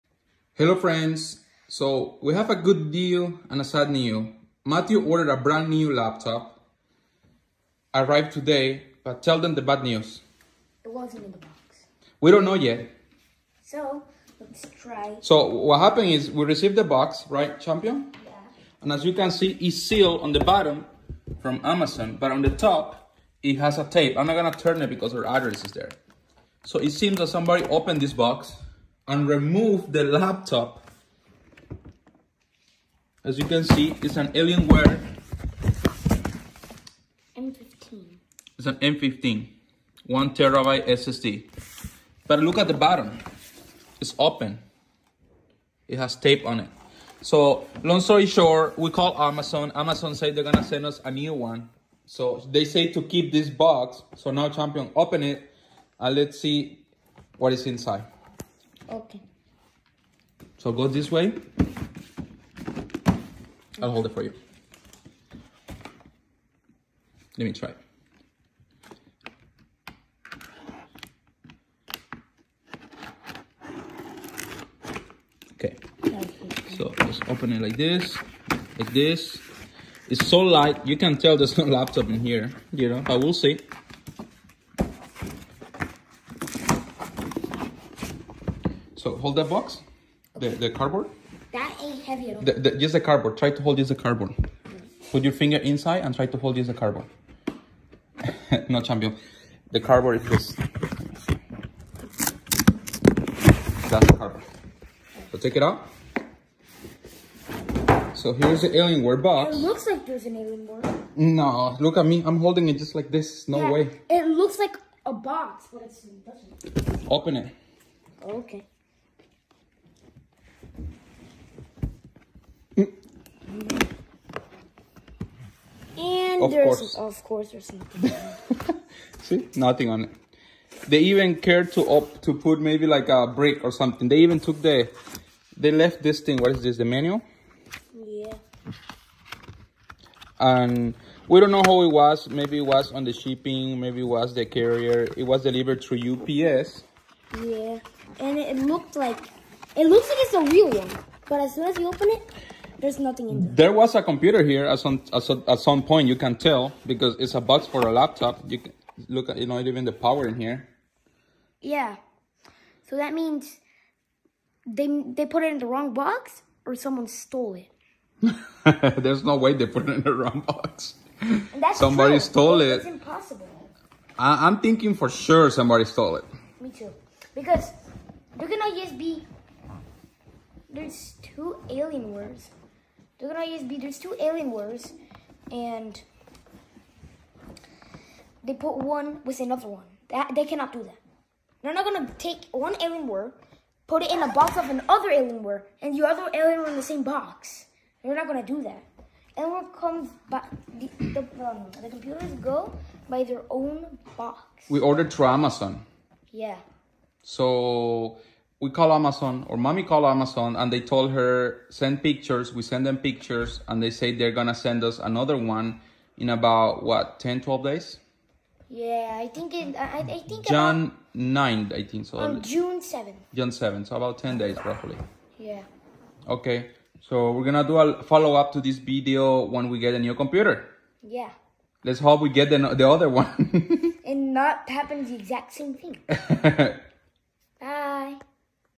☹️Guess what we received from Amazon☹️
Sadly we ordered a new Alienware laptop trough Amazon and Received an empty box. Thankfully we called Amazon they asked for pictures we sent them and they said that will be sending a brand new laptop soon. Yay 😀 will keep you informed once we get the new one. ✌️